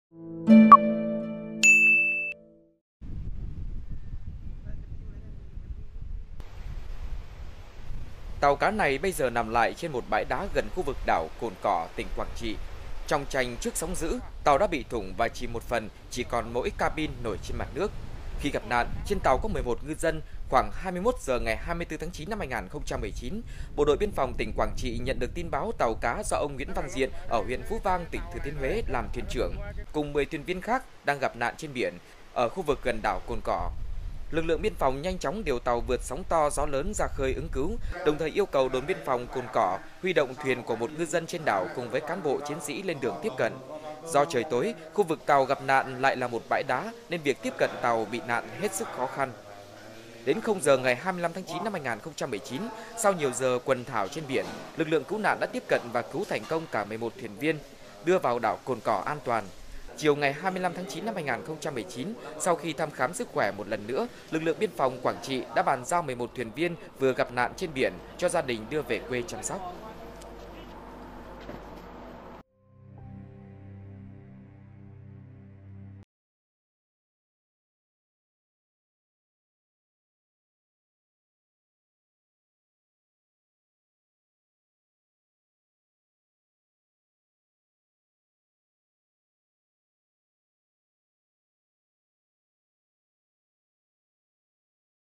Nửa đêm chật vật cứu 11 ngư dân mắc kẹt trên bãi đá giữa biển
11 ngư dân trên tàu cá bị mắc kẹt trên một bãi đá gần khu vực đảo Cồn Cỏ, tỉnh Quảng Trị. Tàu đã bị thủng và chìm một phần, chỉ còn cabin nổi trên mặt nước.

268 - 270 Nguyễn Đình Chiểu, Phường 6, Q.3, TP HCM